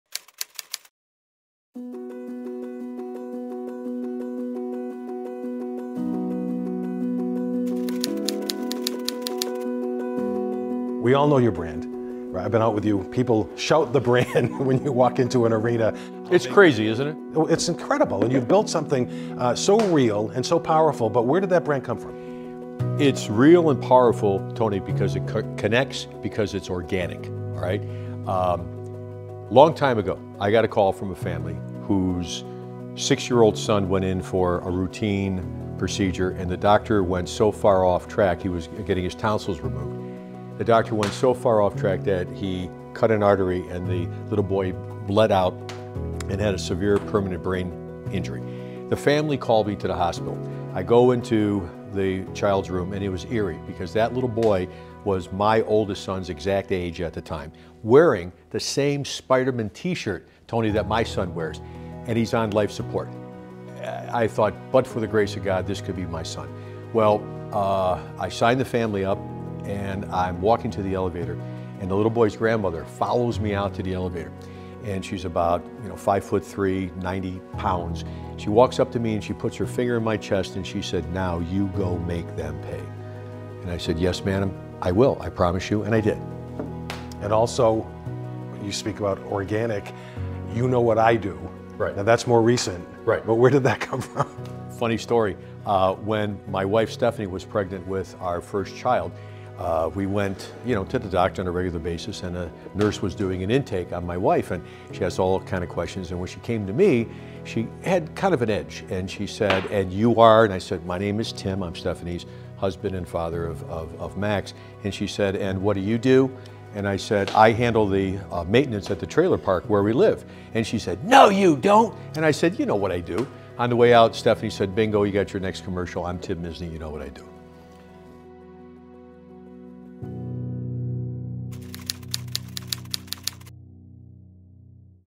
Untold - The Stories Behind Tim Misny
Untold Stories of Tim Misny, interviewed by Tony Zarrella.